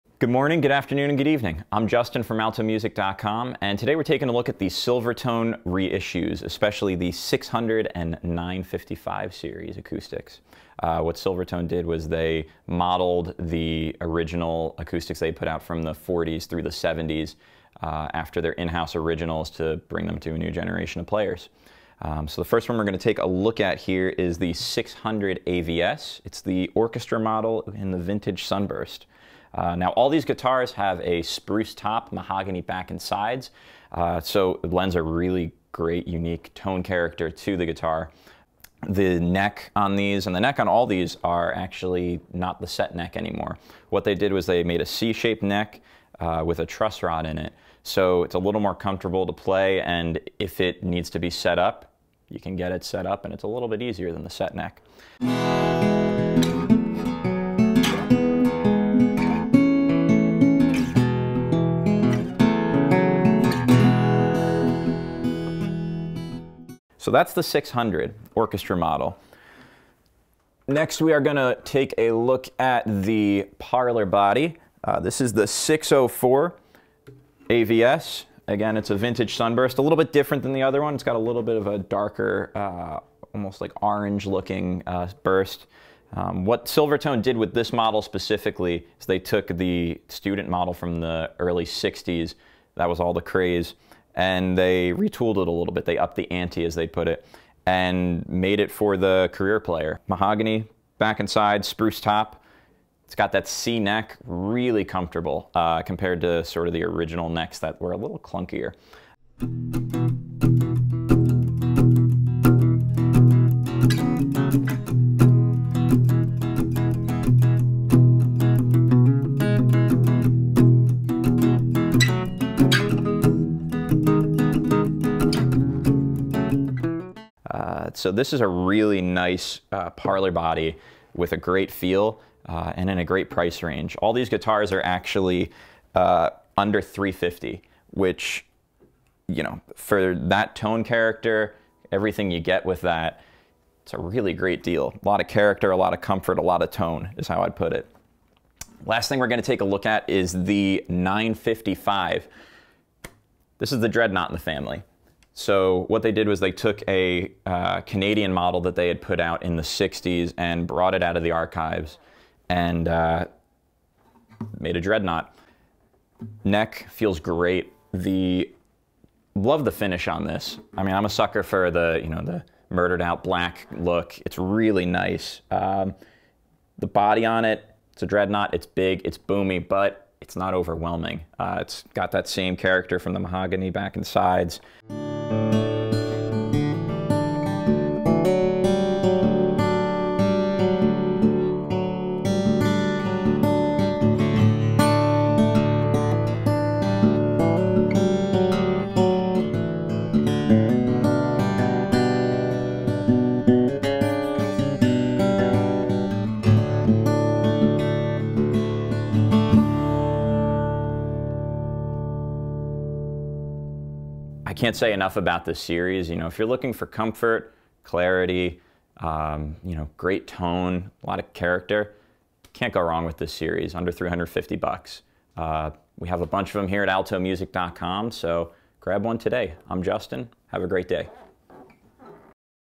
Silvertone Acoustic Guitars (Gear Rundown)
Justin from AltoMusic.com gives a quick rundown of the new Silvertone 600AVS, 604AVS, and 955 reissues now available at AltoMusic.com!